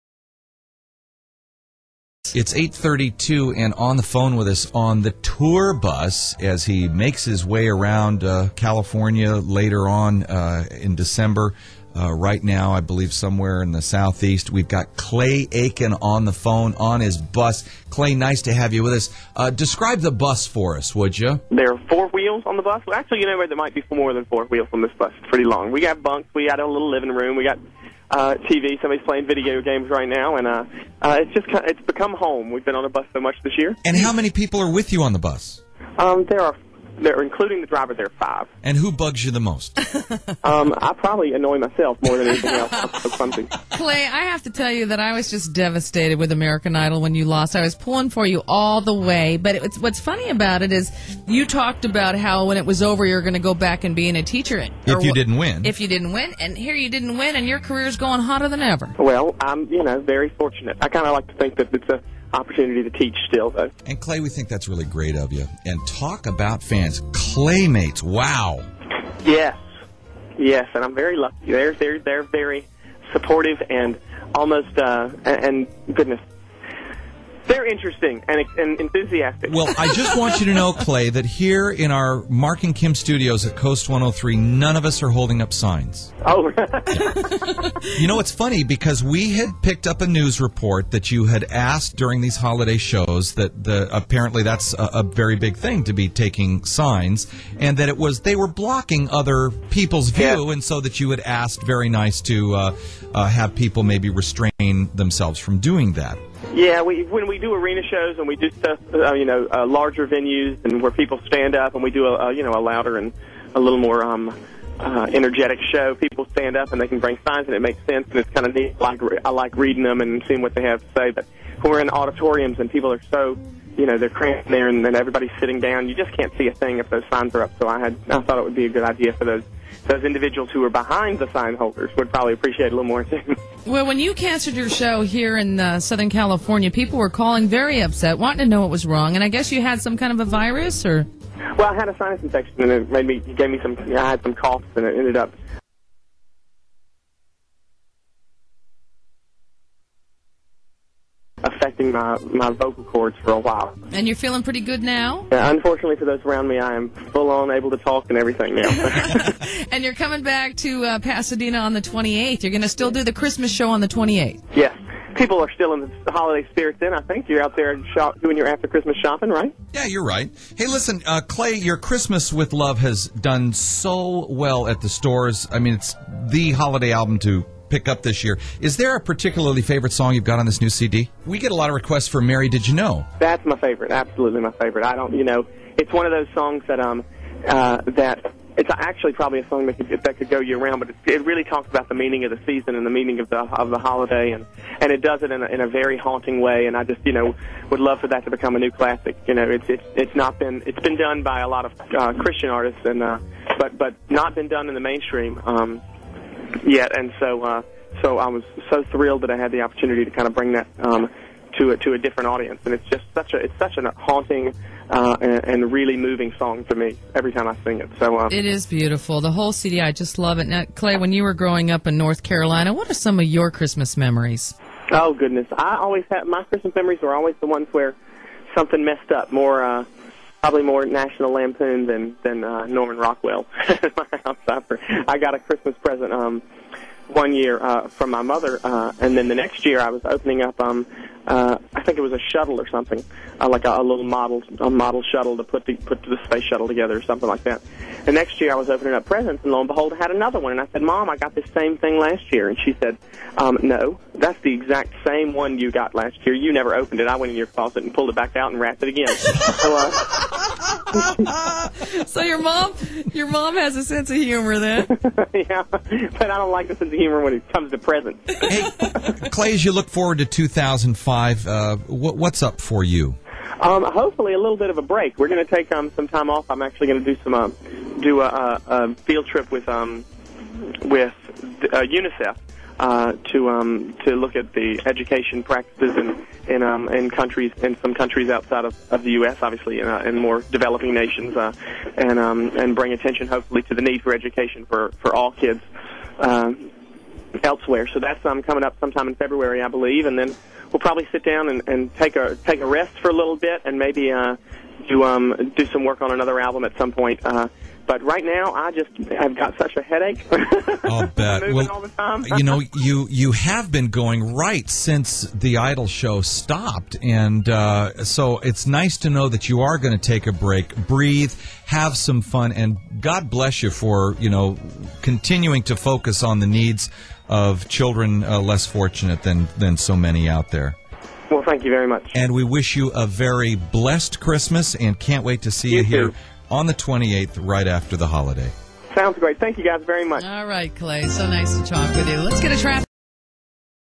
12-17-04 Clay Aiken Radio Interview with KOST FM 103.5
Clay did this interview with KOST 103.5 FM in Los Angeles, CA to promote his first Christmas tour called the Joyful Noise Tour.